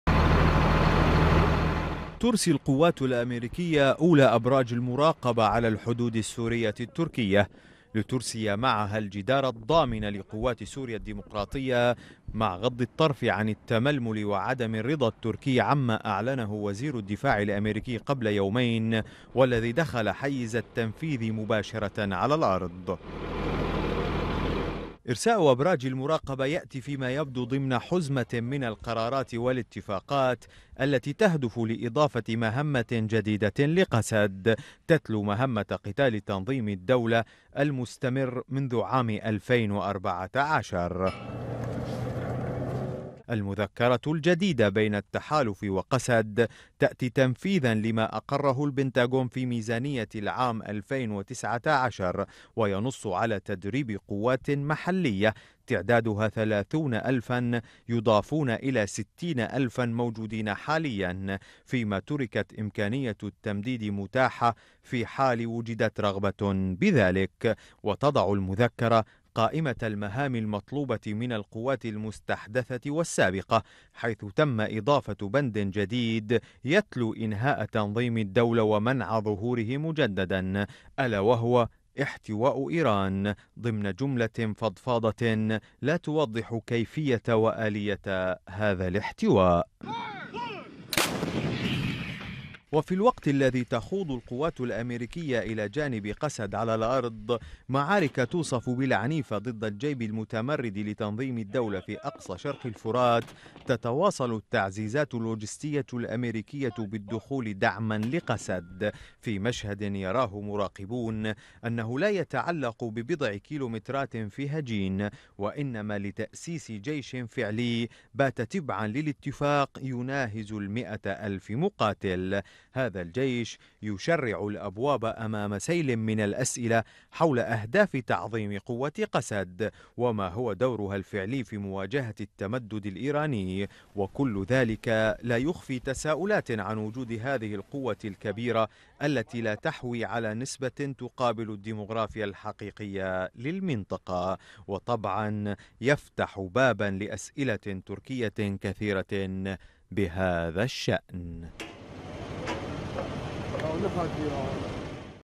واشنطن وقسد .. قصة تحالف جديد تحت بند "احتواء إيران"
بدأتِ الولاياتُ المتحدةُ الأمريكيةُ إنشاءَ نقاطِ المراقبةِ التابعةِ لها على الحدودِ السوريّةِ- التركية، وذلك بعدَ يومينِ من إعلانِ وزيرِ الدّفاعِ الأميركيّ جيمس ماتيس عن هذهِ الخطوة.
ونقلتْ صحيفةُ الشرقِ الأوسط عن قياديٍّ في قوّاتِ سوريا الديمقراطية قولَهُ: إنّهُ تمَّ توقيعُ مذكرةِ تفاهمٍ معَ التحالفِ الدّوليّ لمدةِ عامٍ تتضمّنُ تدريبَ ثلاثينَ ألفَ مقاتلٍ وإضافةَ مهمةٍ جديدةٍ لقسد تتمثلُ باحتواءِ إيران.